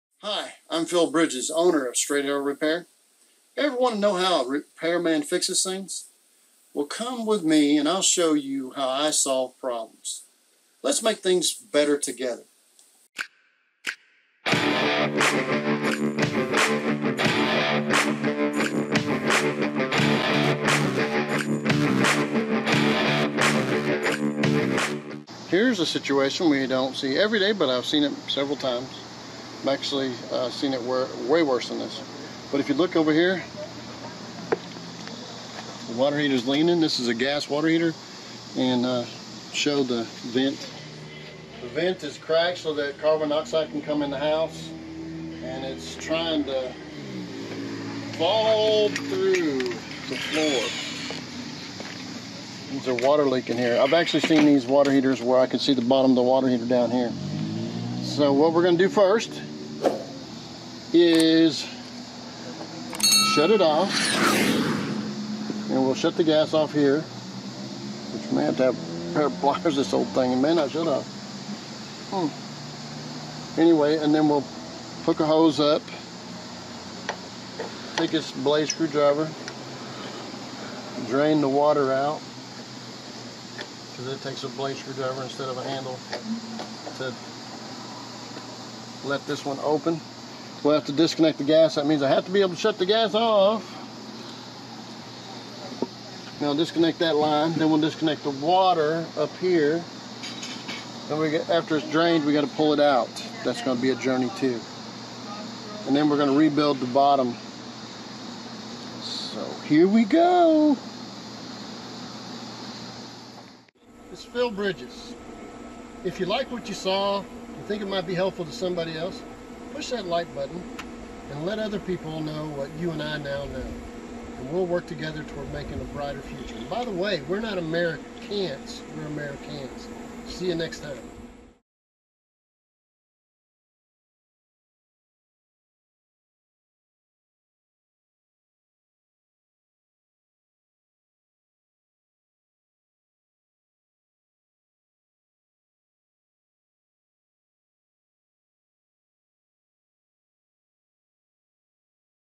Gas Water Heater Problems
Problems with a gas water heater, not something you see everyday. I've seen way worse.

⏱️⏱️Chapters⏱️⏱️
00:00 Follow along with a handyman
00:25 Unusual gas water heater problems
00:43 Vent is cracked, letting carbon monoxide into the home
00:56 He's seen the water heater below the floor when there's a major water leak
01:30 Disconnect and drain the water heater
01:53 Be an American, not an American't

Transcript:
Hi, I'm Phil Bridges, owner of Straight Arrow Repair. Ever want to know how a repairman fixes things? Well come with me, and I'll show you how I solve problems. Let's make things better together.

Here's a situation we don't see every day. But I've seen it several times. I'm actually seen it where, way worse than this. But if you look over here, water heaters lean in.

This is a gas water heater and show the vent. The vent is cracked so that carbon dioxide can come in the house and it's trying to fall through the floor. There's a water leak in here. I've actually seen these water heaters where I could see the bottom of the water heater down here. So what we're going to do first is shut it off and we'll shut the gas off here.

Which we may have to have a pair of plyers this ole thing. It may not shut off. home anyway, and then we'll hook a hose up, I think this is a balde screwdriver, drain the water out because it takes a blade screwdiver instead of a handle to let this one open, we'll have to disconnect the gas. That means I have to be able to shut the gas off.

And I'll disconnect that line. Then we'll disconnect the water up here. That way get after drained. We've got to pull it out. That's going to be a journey to and then we're going to rebuild the bottom. So here we go.

This is Phil Bridges, if you like what you saw and think it might be helpful to somebody else, push that like button and let other people know what you and I know, and we'll work together toward making a brighter future. By the way, we're not Amera-Can'ts, we're Americans.